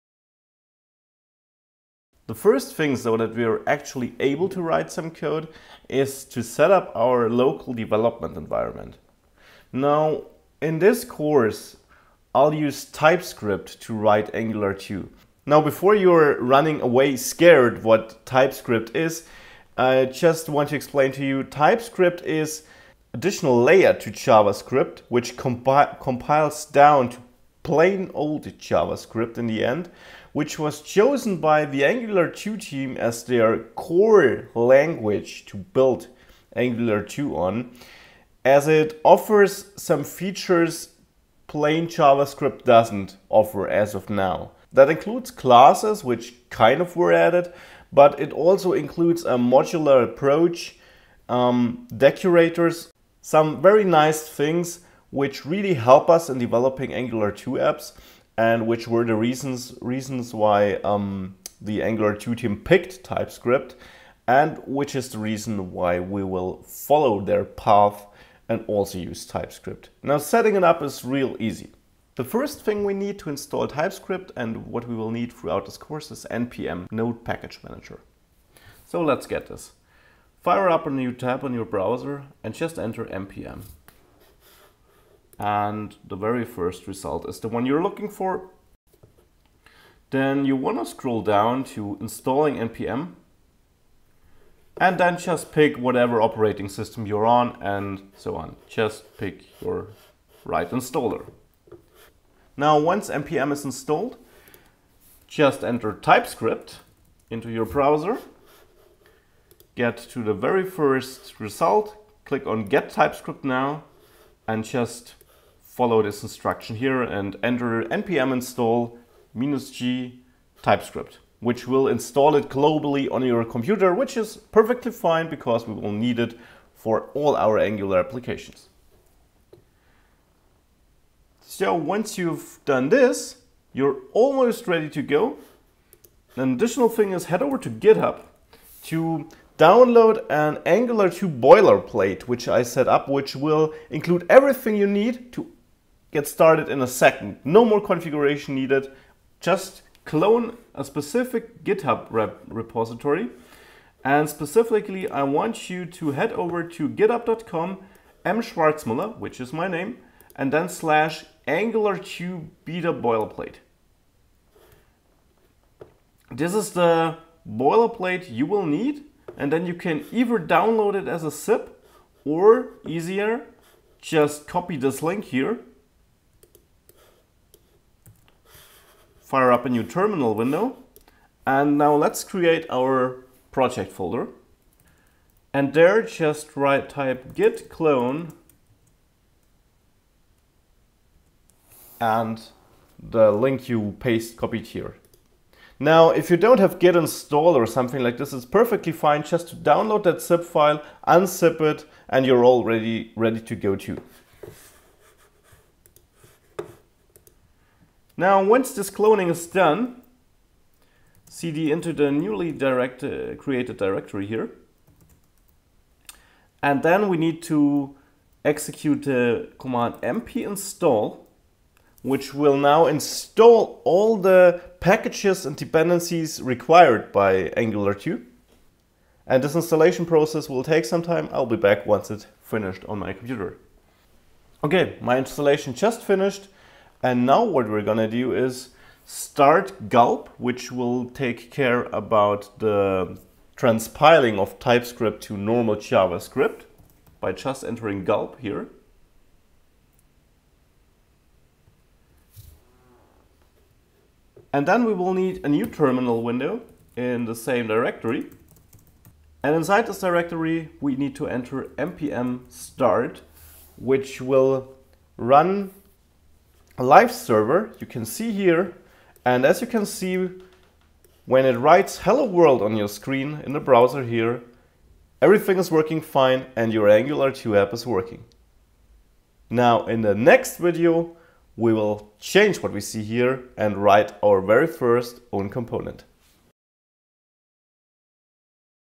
Angular 2 Tutorial (2016) - Setting up the Development Environment
Want much more Angular? Have a look at my bestselling course to learn the latest version of Angular

Angular 2 Tutorial which guides you how to setup your Angular 2 development environment.

You may learn more about the Angular 2 Development Environment in this article

This course will teach you all the fundamentals of the current Angular 2 Beta and will prepare you to write actual Angular 2 web applications.

We will cover Directives, Components, Template Syntax, Two-Way-Data Binding, Services, HTTP (GET and POST) and much more!

In order to get the most out of this course be sure to use my prepared GitHub Repository as a Boilerplate

Have fun and please leave a like or comment if you like (or dislike) this video or this series!